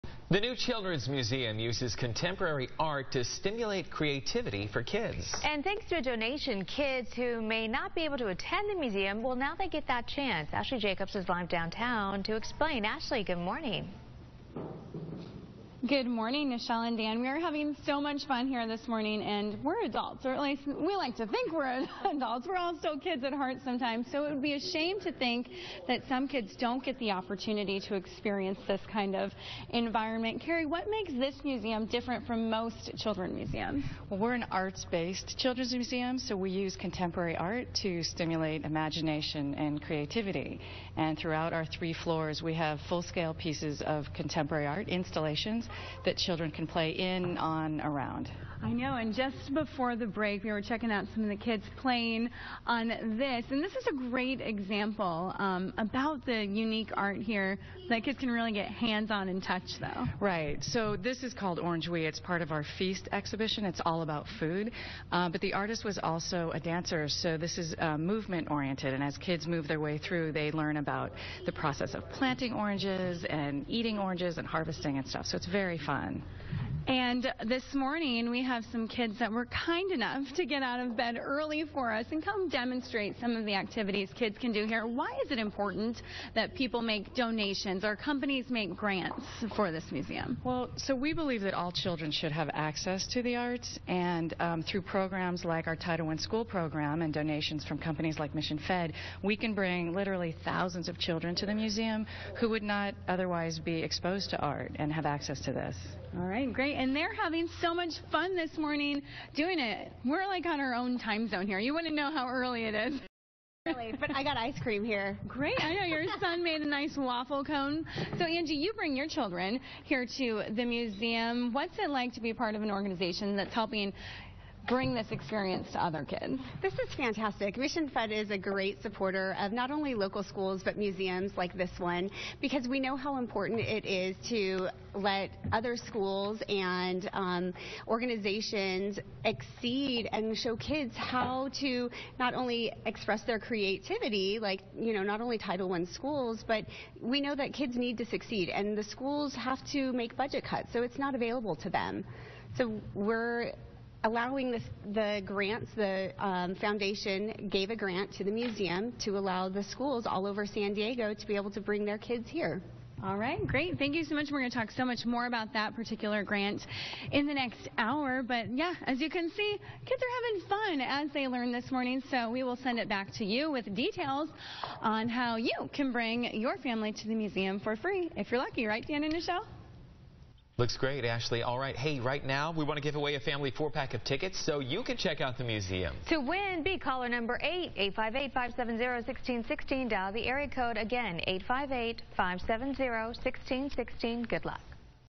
Mission Fed Partners with New Children's Museum Part 1
The New Children’s Museum is a new model of children’s museum whose mission is to stimulate imagination, creativity and critical thinking in children and families through inventive and engaging experiences with contemporary art.

The New Children’s Museum has partnered with Mission Federal Credit Union to make it possible for the museum to continue to bring engaging art-making experiences to those that might not otherwise have the opportunity.